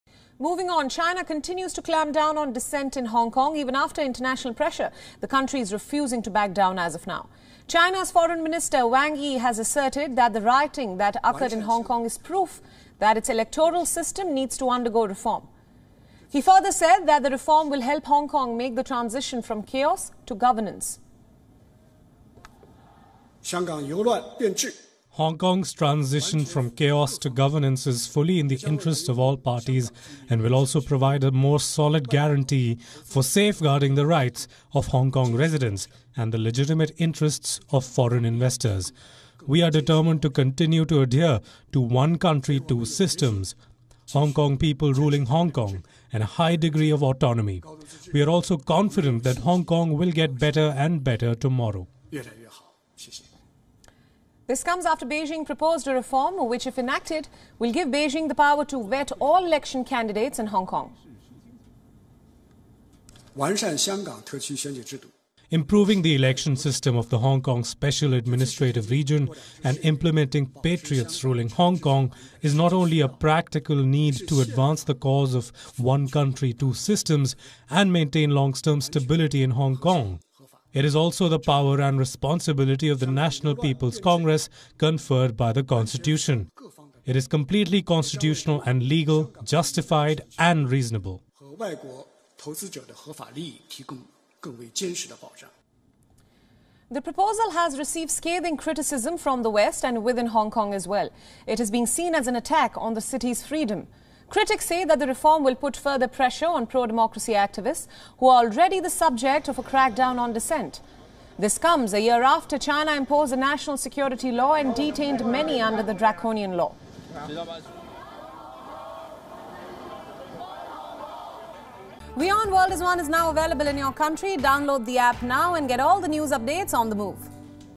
Chinese Foreign Wang Yi Minister defends Hong Kong reform | Electoral System | English News | WION
China continues to clampdown on Hong Kong even after international pressure. Foreign Minister Wang Yi has asserted that the rioting which occurred in Hong Kong is proof that its electoral system needs to undergo reform.